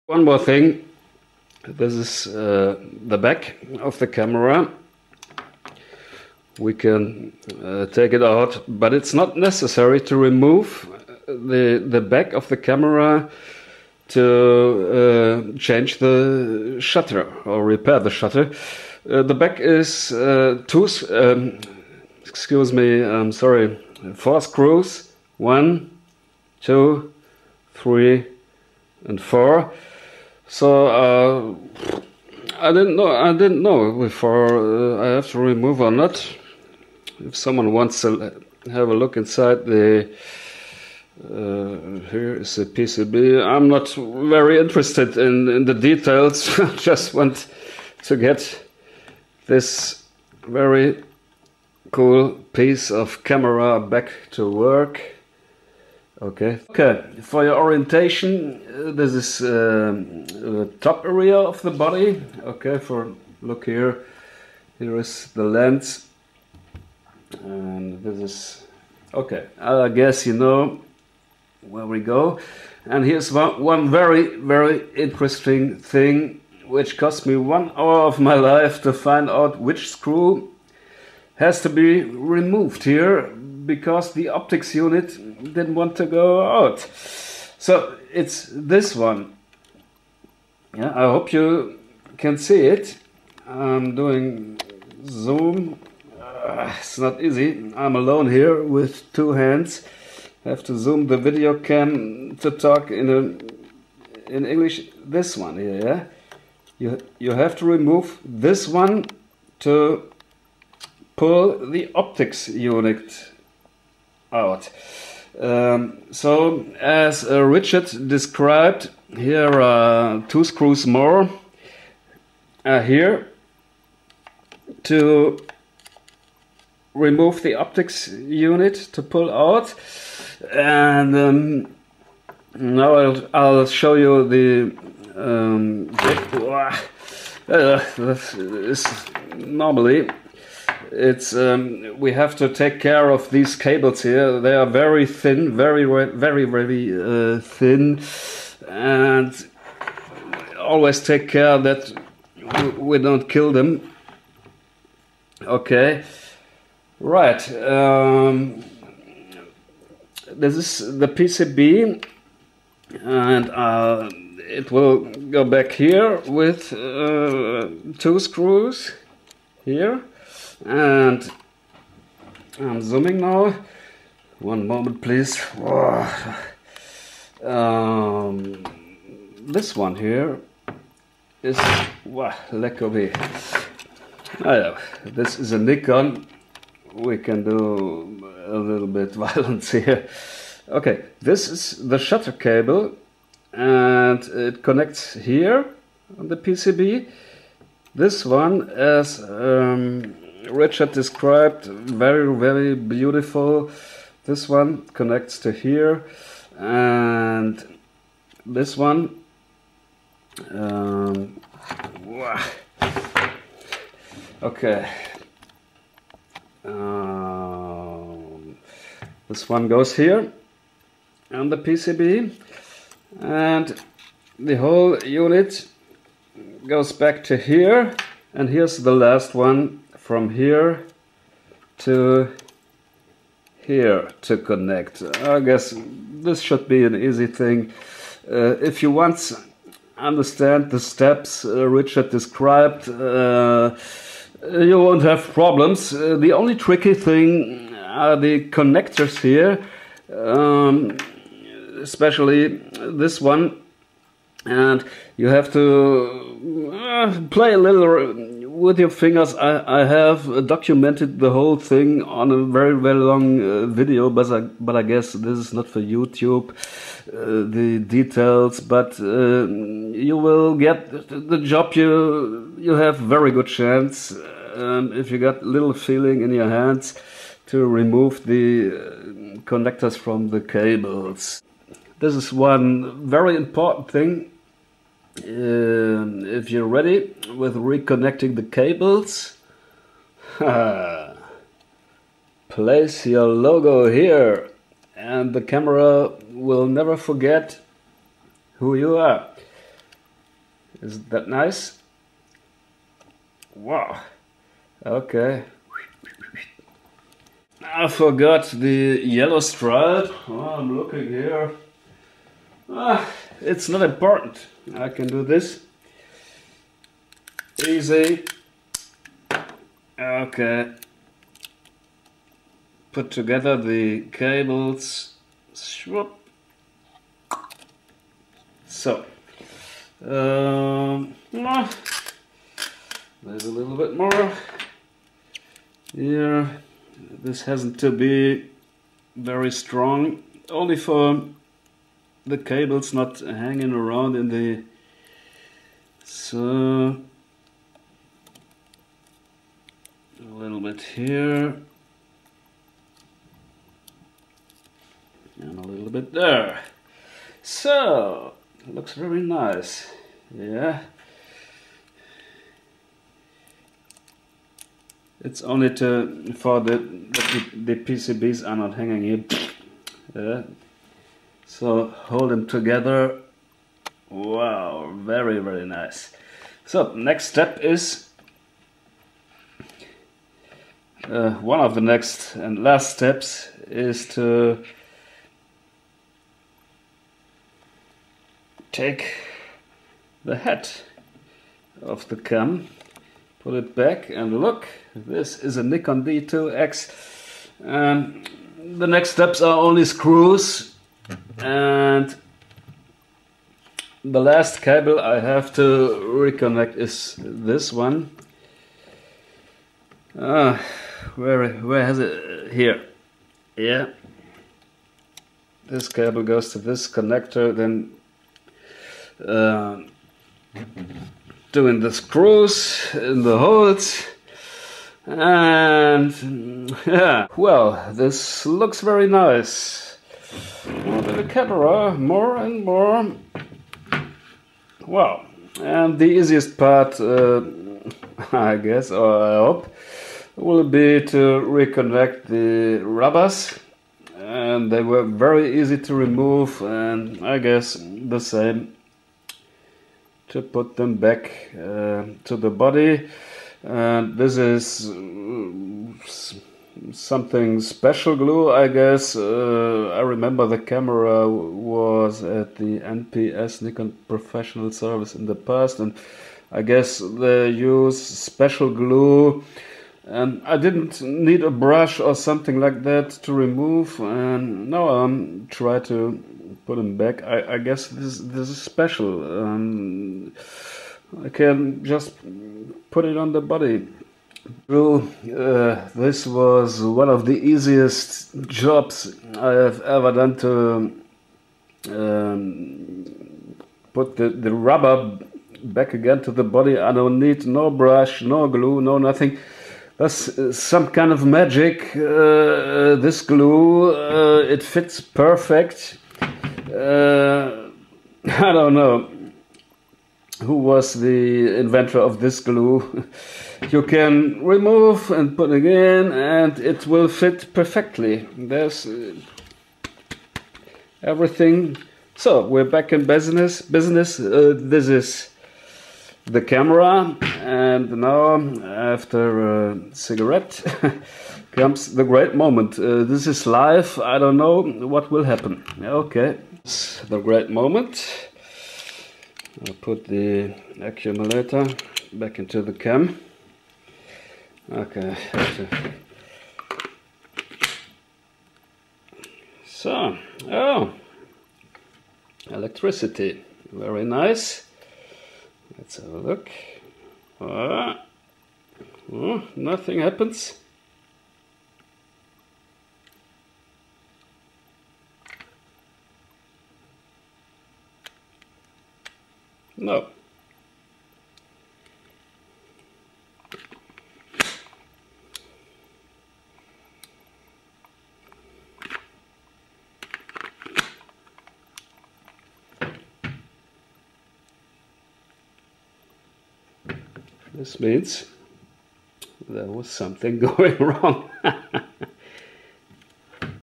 Nikon D2X selfmade shutter repair manual PART 2/3
How I dissasembled a Nikon D2X and got a broken shutter  and the whole camera back to life